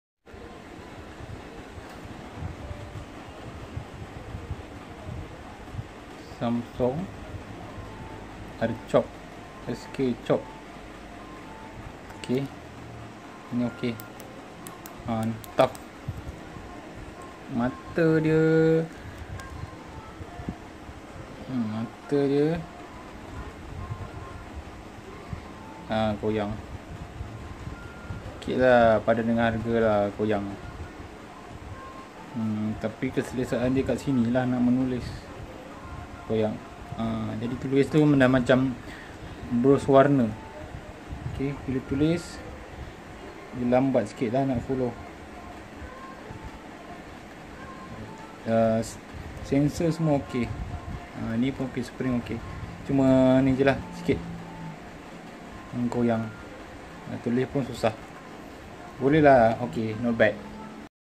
Note 5 OEM Pen stylush
Review pen note 5 beli dari shoppe dgn RM 30 je...